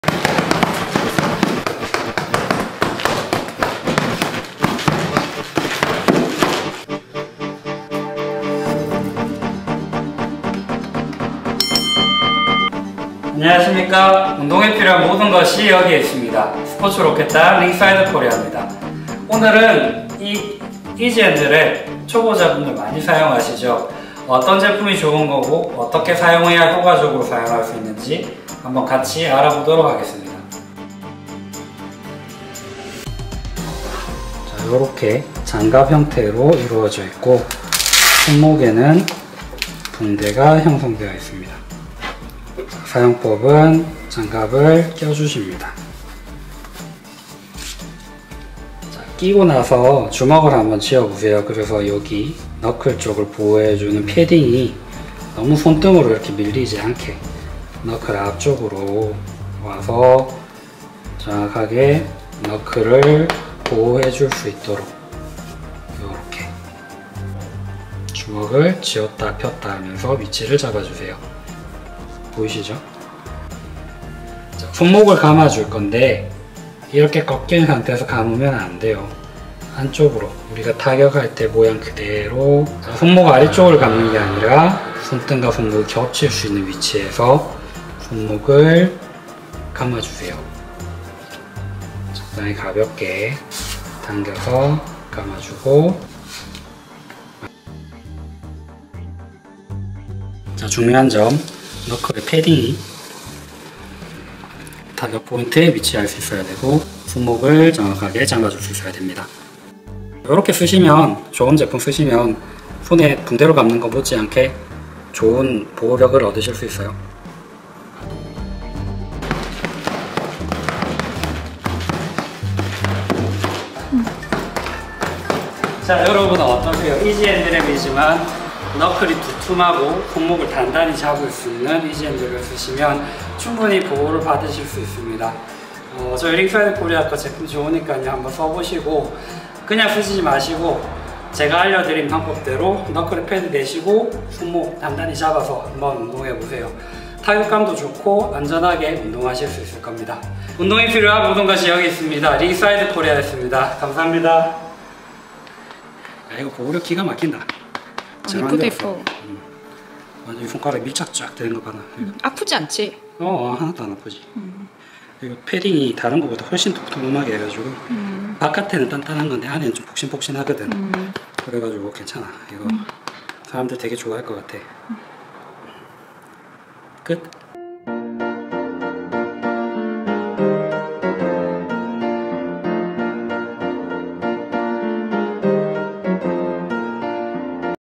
초보자도 쉽게 사용하는 복싱 이지 핸드랩 제대로 사용하는 법 추천 리뷰 사용법 감는법 정권 보호법
안녕하세요 링사이드 코리아입니다.
오늘 소개해 드릴 제품은 복싱 이지 핸드랩입니다.

간편하게 사용하는 초보자들을 위한 제품처럼 여겨지지만,

좋은 제품으로 제대로 사용하신다면,
쉽고 빠르게 손을 안전하게 보호하는 효과를 얻으실 수 있습니다.

링사이드 코리아 이지핸드랩으로,
안전하게 즐겁게 운동하세요^^